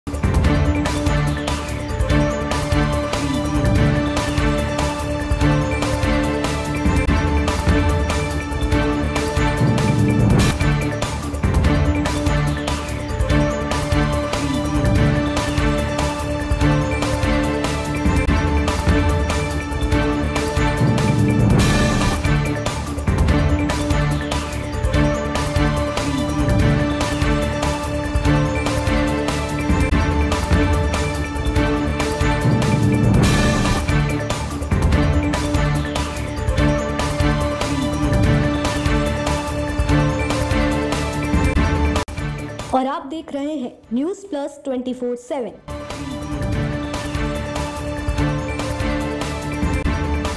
BREAKING KANPUR कानपुर-पेट्रोल पंप पर लूट करने वाले शातिरों से पुलिस की मुठभेड़ | CROOK ARREST | NEWS
BREAKING KANPUR कानपुर-पेट्रोल पंप पर लूट करने वाले शातिरों से पुलिस की मुठभेड़